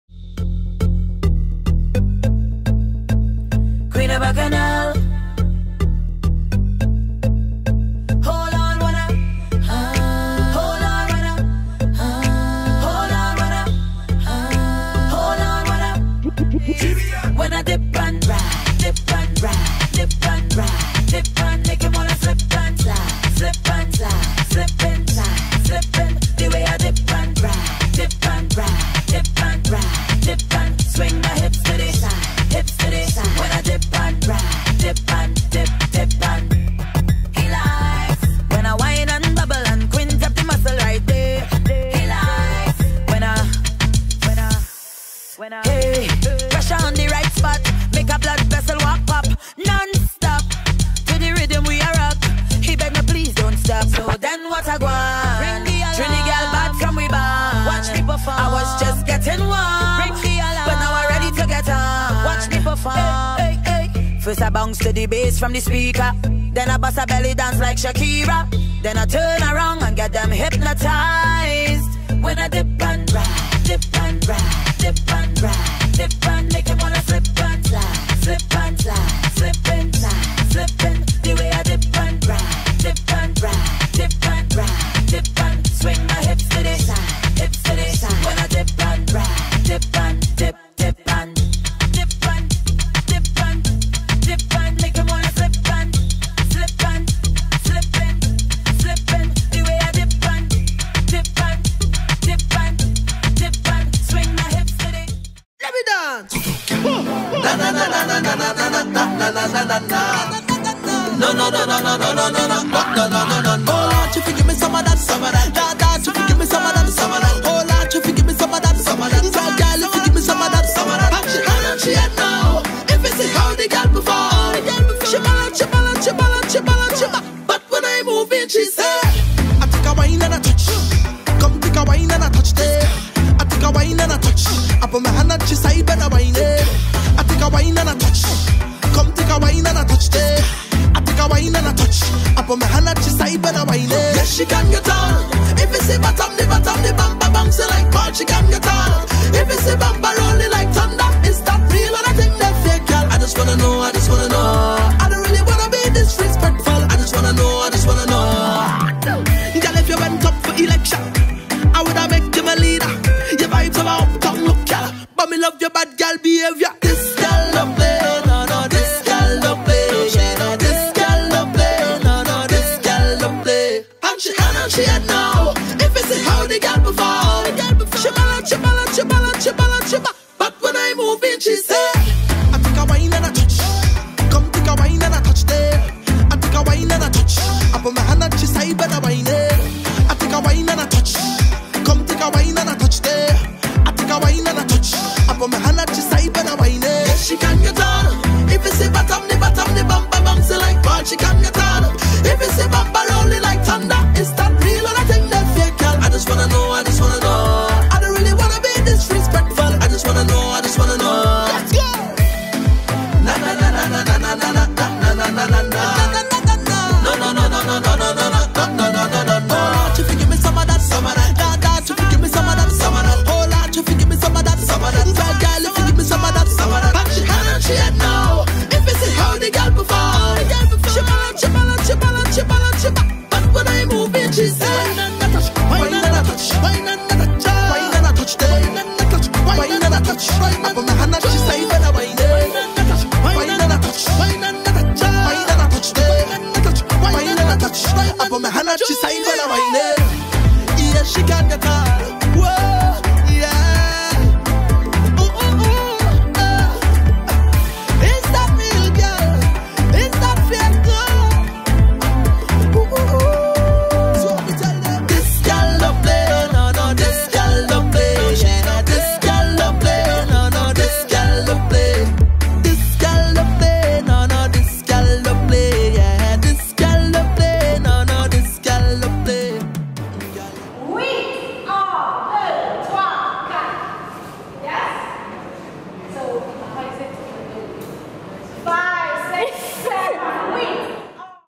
Guadeloupe Soca Master Class with La Shaun Prescott 2nd Edition
Caribbean Ikon and La Shaun Prescott of Elle NYTT present the 2nd edition of the Soca Master Class in Guadeloupe held on Sunday 18th October 2015.

Thanks to Naika Pichi Ayers of Caribbean Ikon, Johana Morvan of Gossip Curl and Mario Guiolet for organizing the event.

Thanks to Katherine "Katie C" Carrera of Social K Media for assisting with instruction and videography.

Special thanks to all the participants and congratulations to the winner: Michael Jannas Alias Jek Unik.

New York: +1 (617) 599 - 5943
Trinidad: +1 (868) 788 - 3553